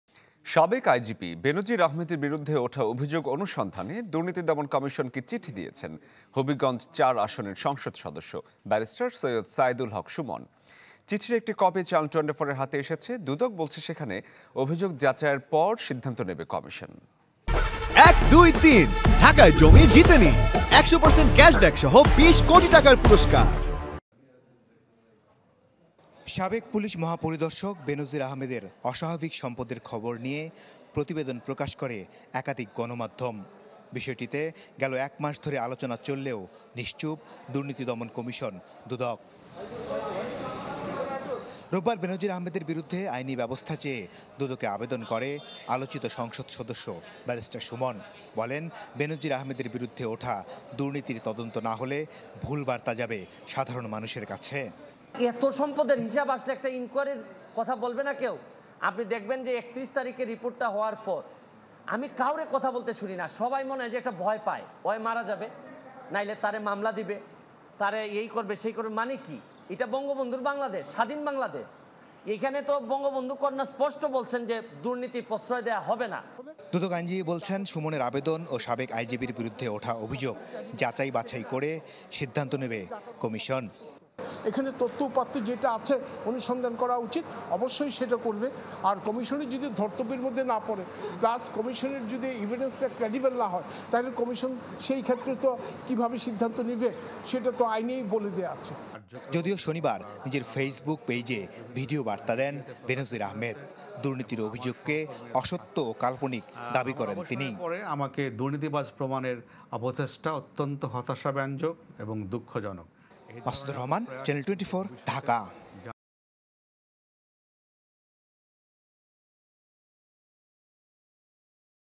বেনজীরের দুর্নীতি অনুসন্ধানে দুদকে চিঠি দিলেন ব্যারিস্টার সুমন | Benazir Ahmed | Barrister Sumon
সাবেক আইজিপি বেনজীর আহমেদের বিরুদ্ধে ওঠা অভিযোগ অনুসন্ধানে দুর্নীতি দমন কমিশনকে চিঠি দিয়েছেন, হবিগঞ্জ-৪ আসনের সংসদ সদস্য, ব্যারিস্টার সৈয়দ সায়েদুল হক সুমন। চিঠির একটি কপি চ্যানেল-24 এর হাতে এসেছে। দুদক বলছে, অভিযোগ যাচাইয়ের পর সিদ্ধান্ত নেবে কমিশন

»» Office Address:
Bangladesh.